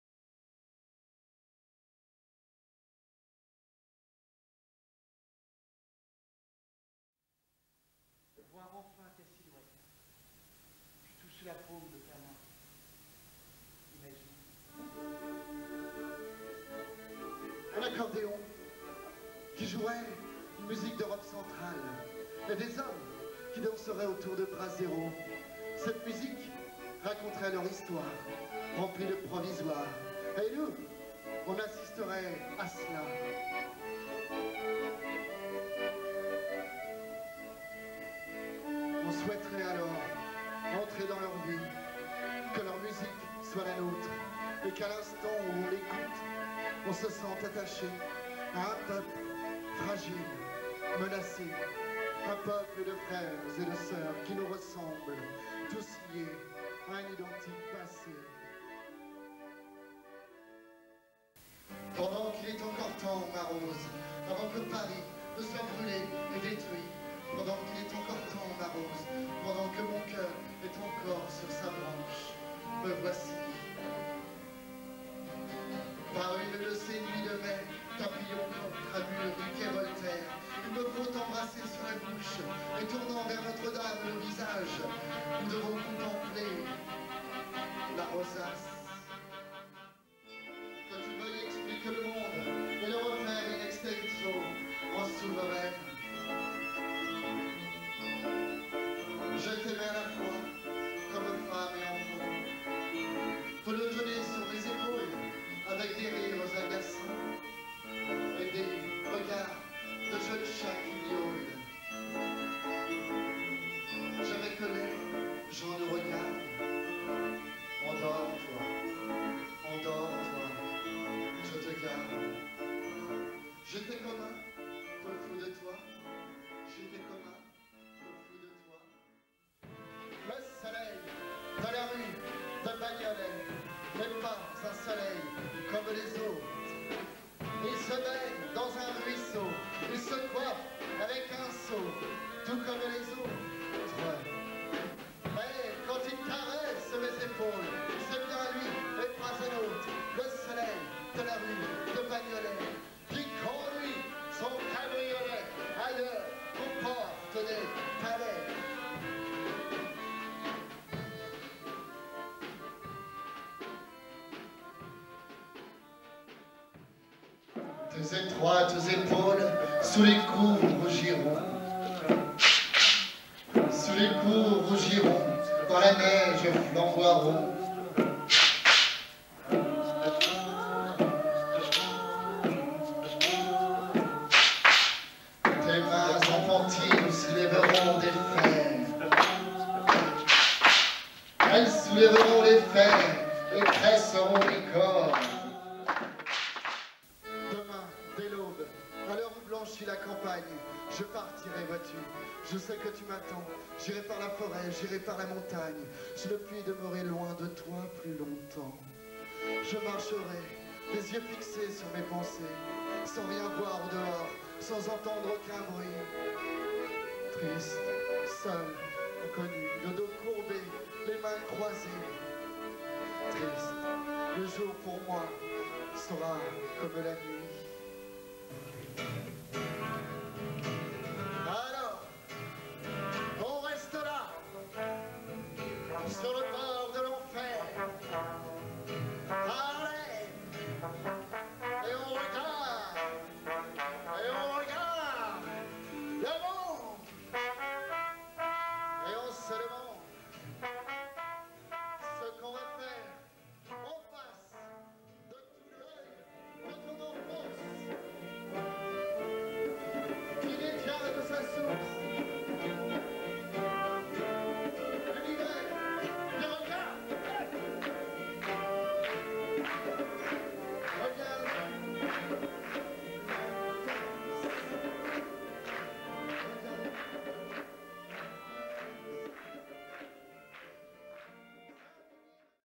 Quartier de plumes
Extraits d'un spectacle de poésies en musiques enregistrées au théâtre de La Roche Sur Yon en 2001. Avec Renaud à la voix, Pierre-Yves à la clarinette, Maud au violon, Mathieu aux percussions, Agathe au piano, Olivier au trombone, Julien à la batterie et Eric à la guitare, l'accordéon, la composition et les arrangements.